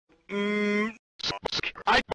My New Outro
I made an outro for my videos.